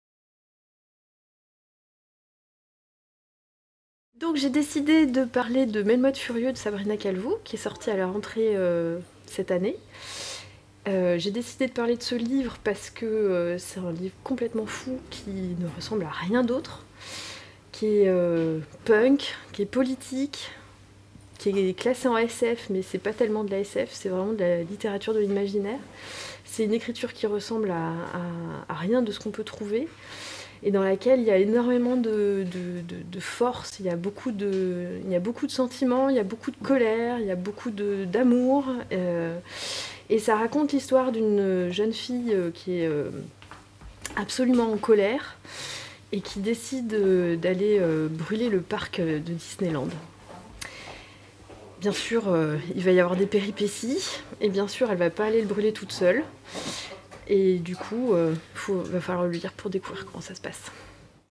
Melmoth Furieux de Sabrina Calvo – Entre nos mains
Léanne, libraire du rayon Science-Fiction, présente Melmoth Furieux de Sabrina Calvo paru aux éditions La Volte.